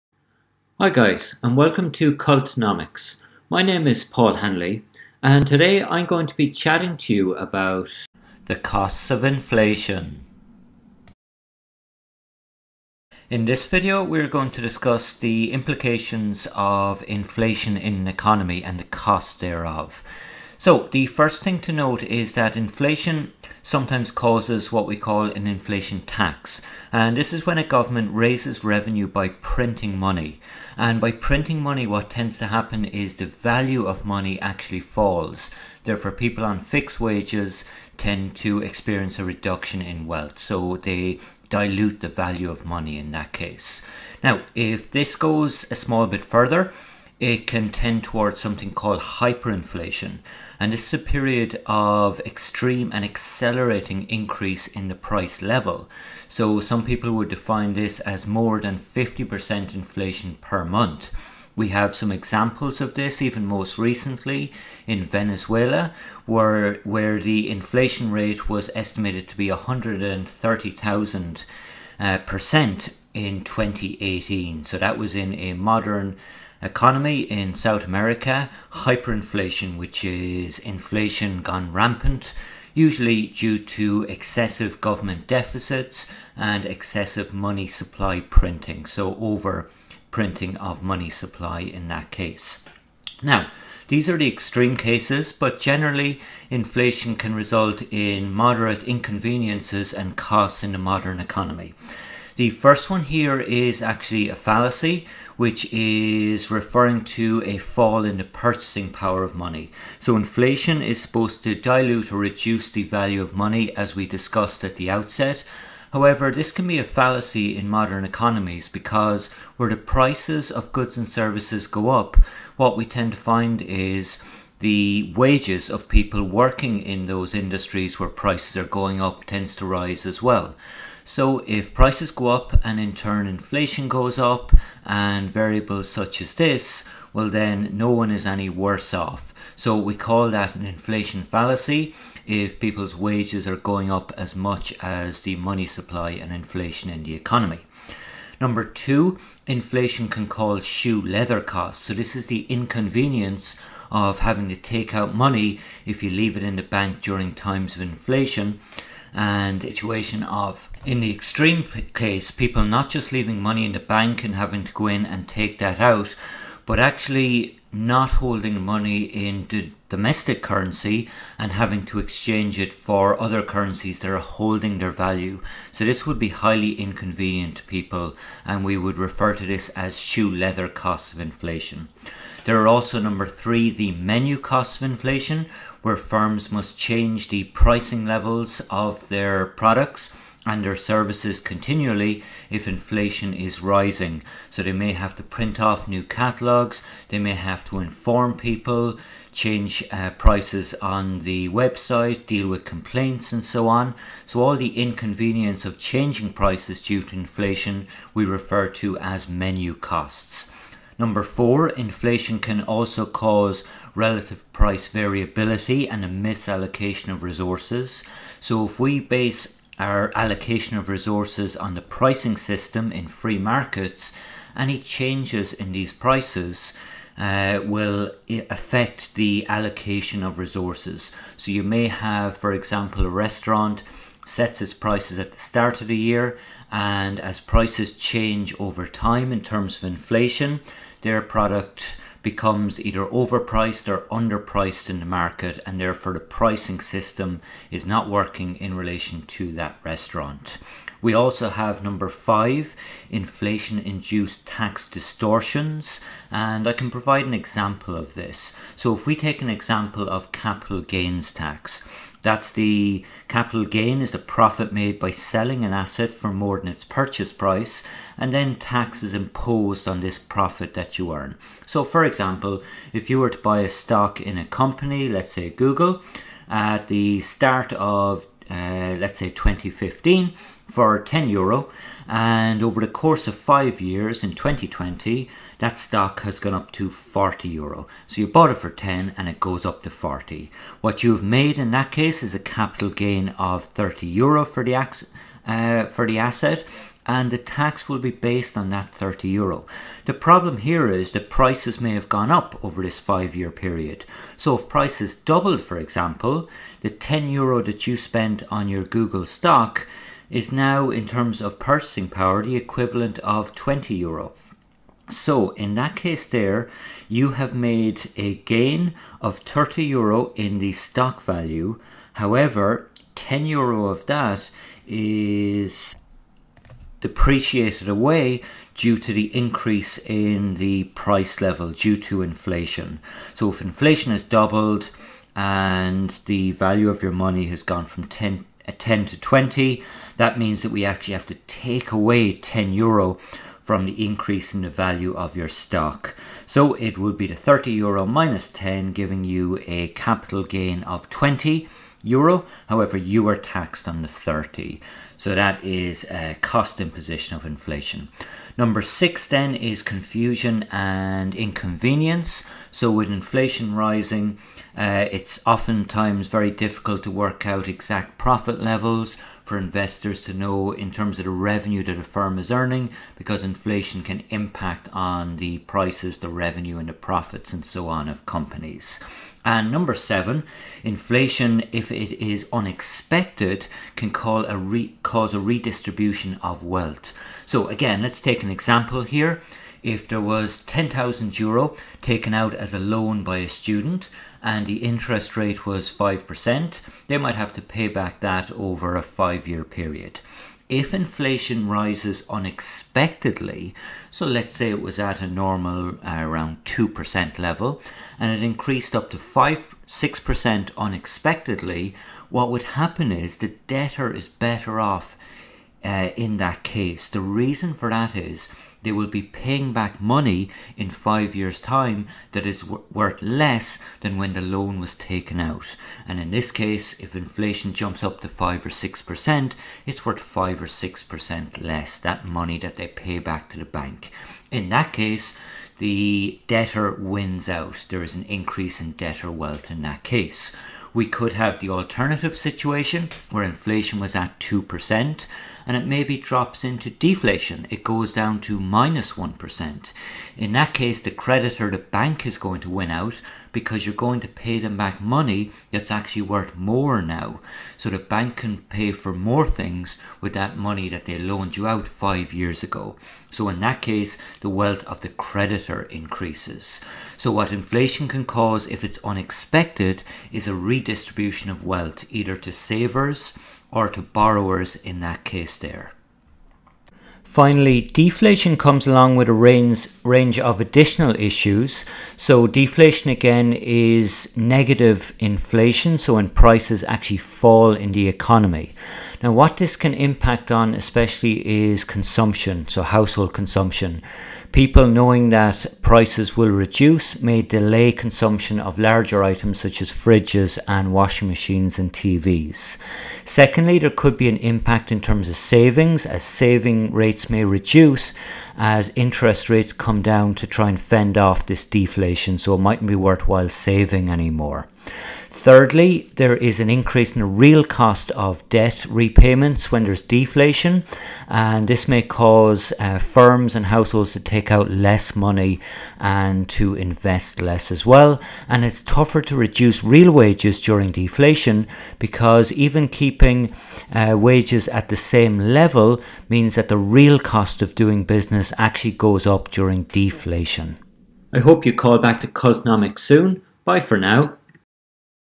9.3 The Costs of Inflation
This video assesses the main costs of inflation to the economy and the individual.